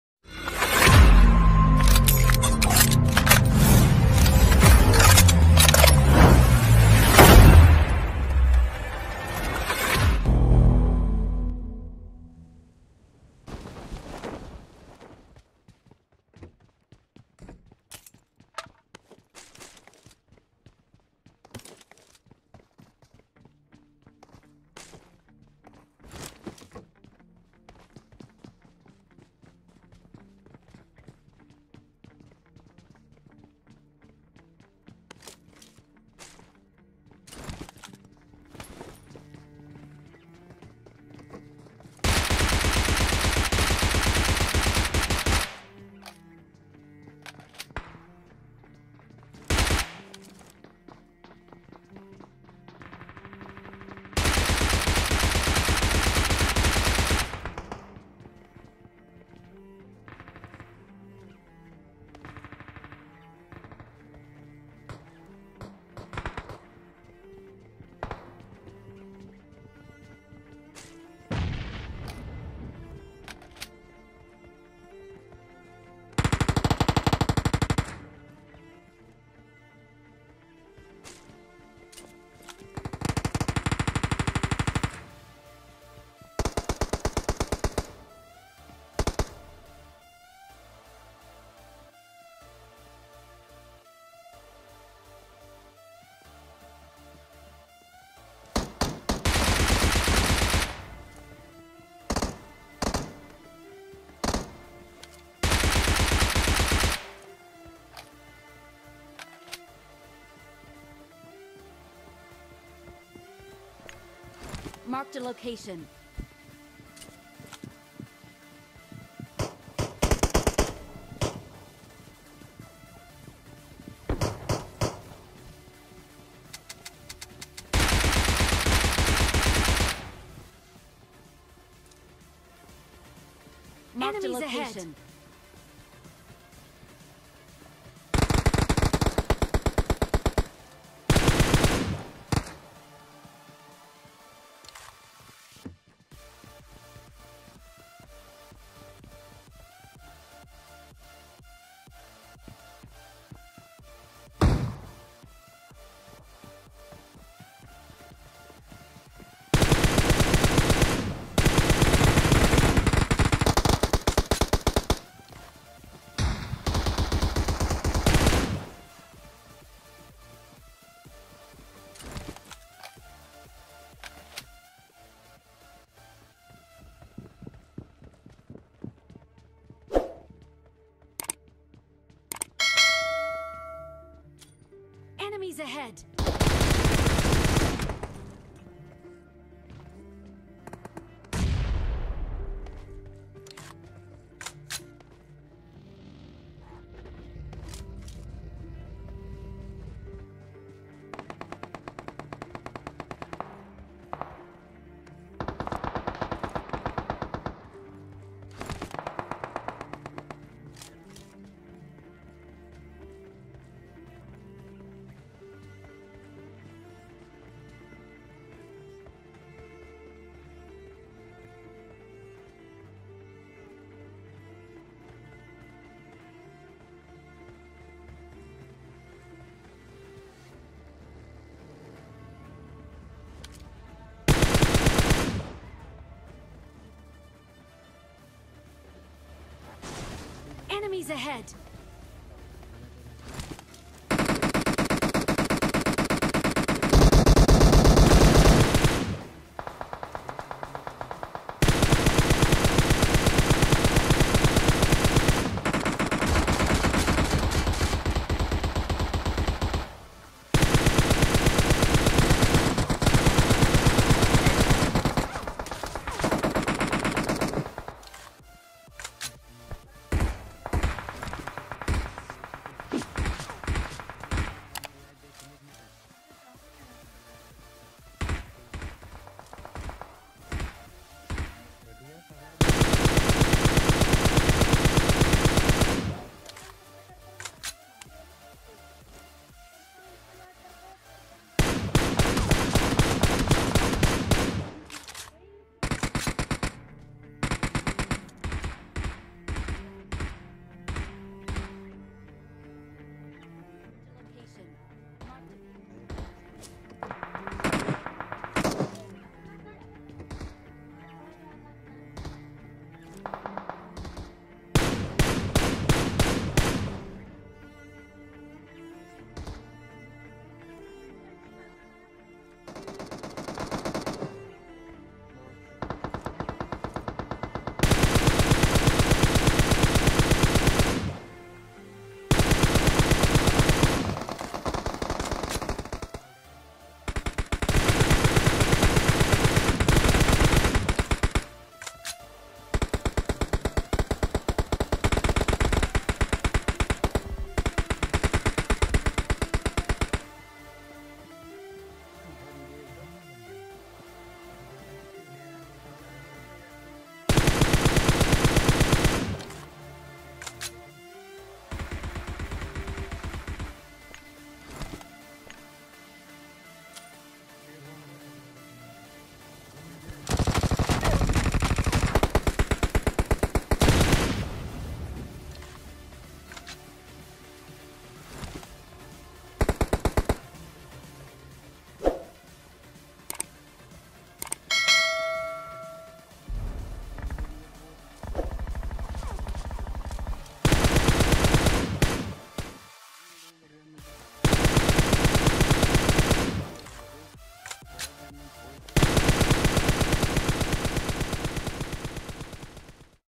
ZONE ME KYA HE SAPERE MARTE MARTE BACHA FULL TABAHI
In this gameplay bhut he khatarnak fight hue kai bar team hamari marte marte bachi next he level ki fight thi see video and enjoy,